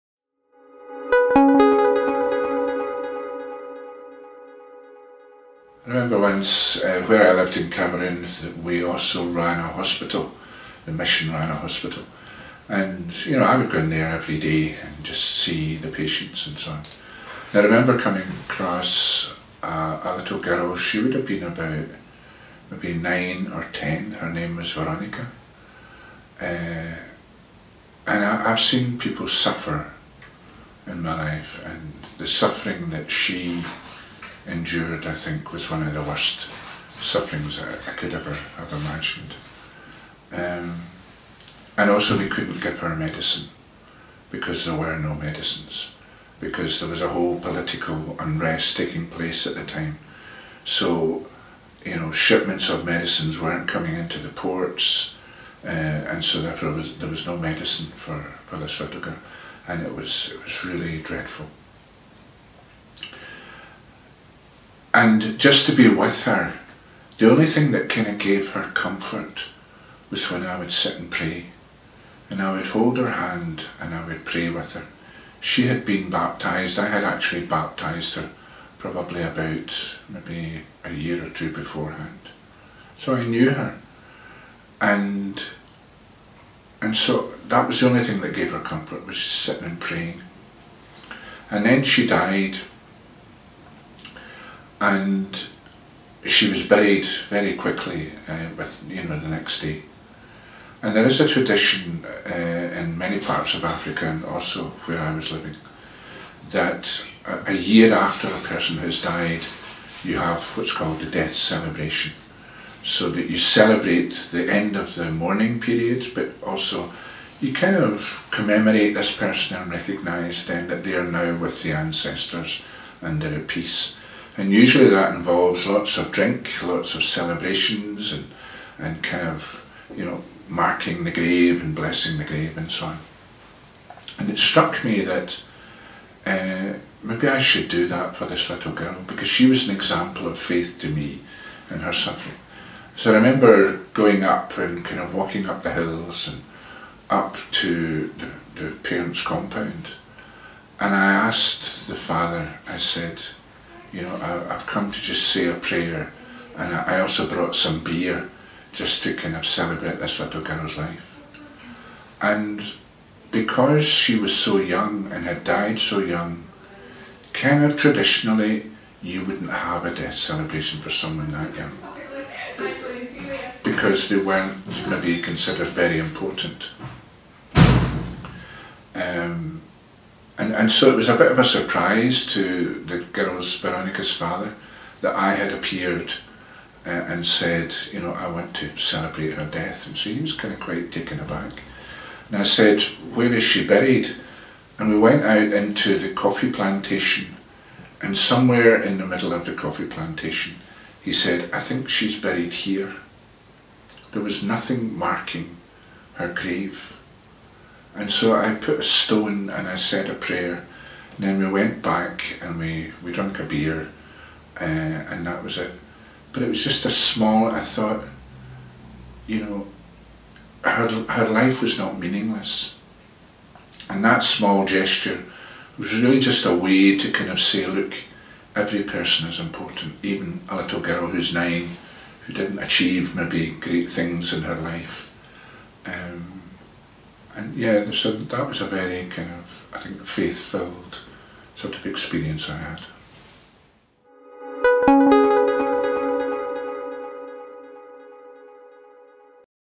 Inspiring faith stories (Fr Vincent Lockhart)
Missio Scotland's National Director, Fr Vincent Lockhart, shares with us an inspiring story of faith that he witnessed while on mission in Cameroon related to a little girl called Veronica